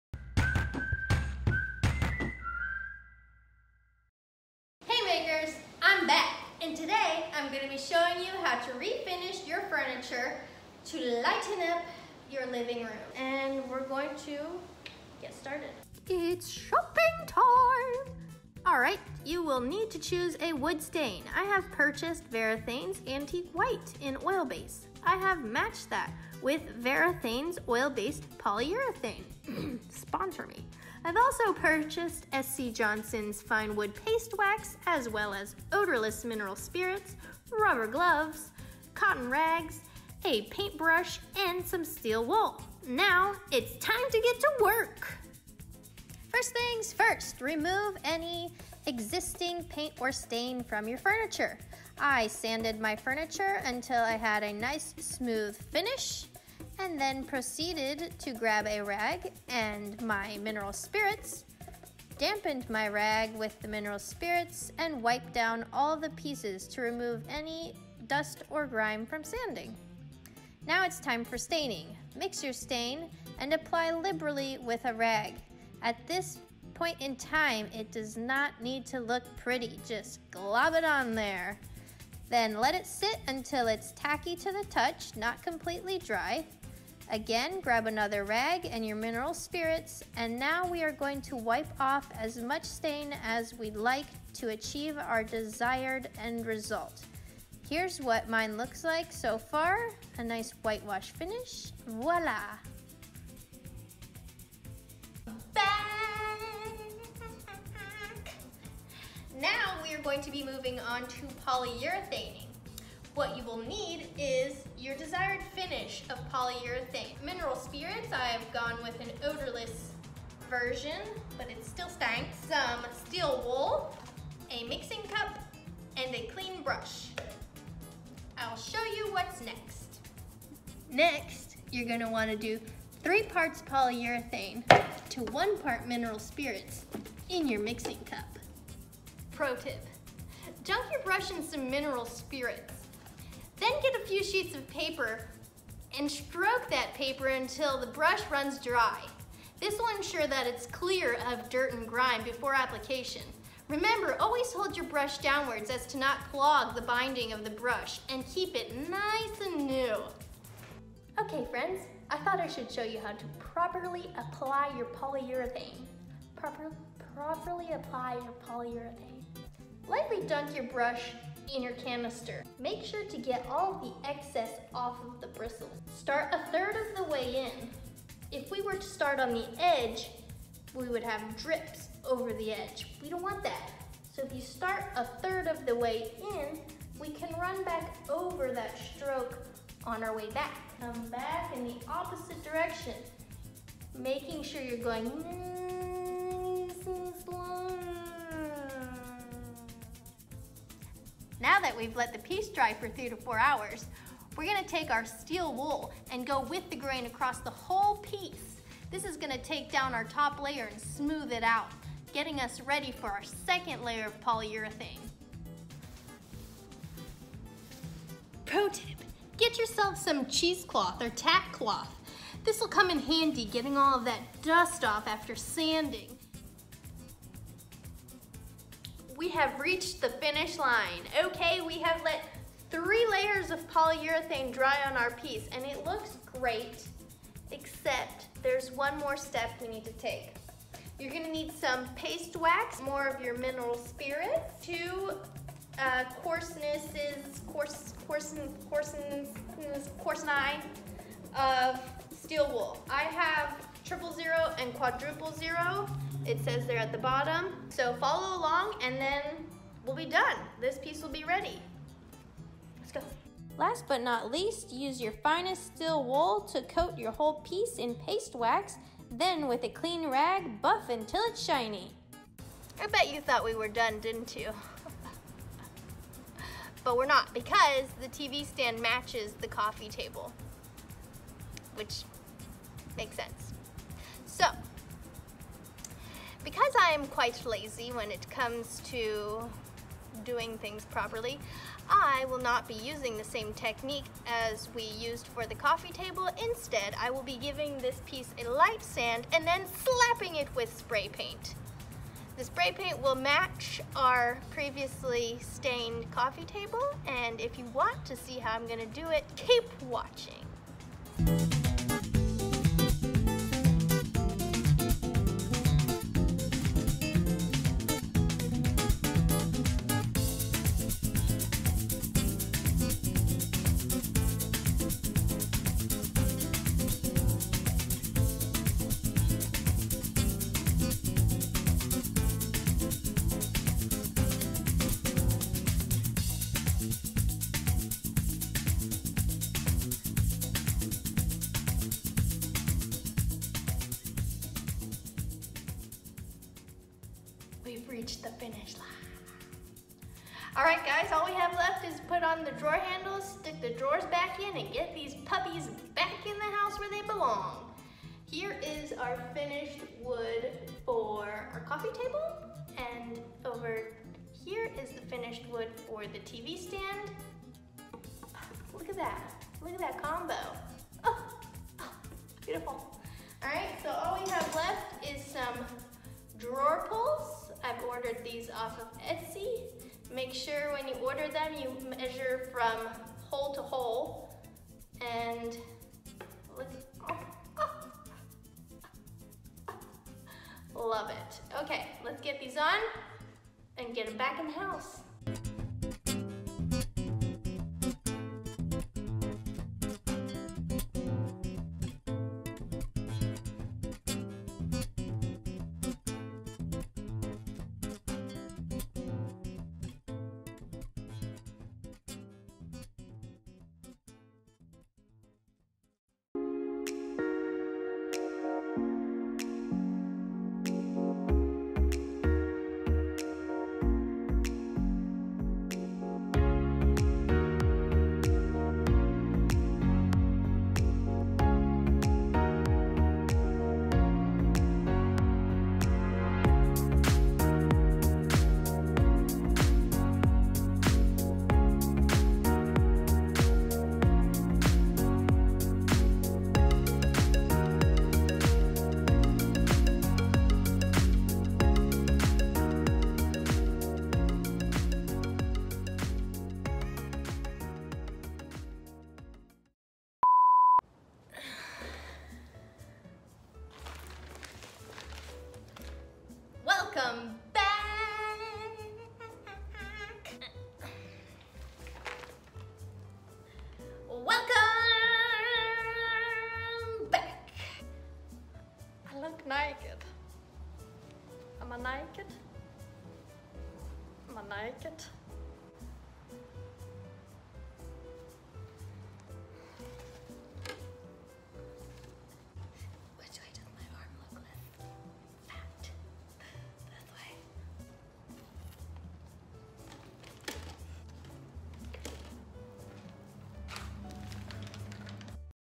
Furniture FLIP | Lightening Dark Furniture DIY
Follow along to see how I transformed my living room without buying any new furniture. With just a few materials from the local hardware store, I slipped my dark furniture to lighten up my living room.

Materials:
• Furniture: existing home furniture $0
• Rust-Oleum Painter's Touch 2X Satin Smokey Beige: Home Depot $3.98
• Varathane Antique White Wood Stain: Home Depot $9.78
• Homax Super Fine Grit Steel Wool: Home Depot $4.27
• SC Johnson Fine Wood Paste Wax: Home Depot $6.47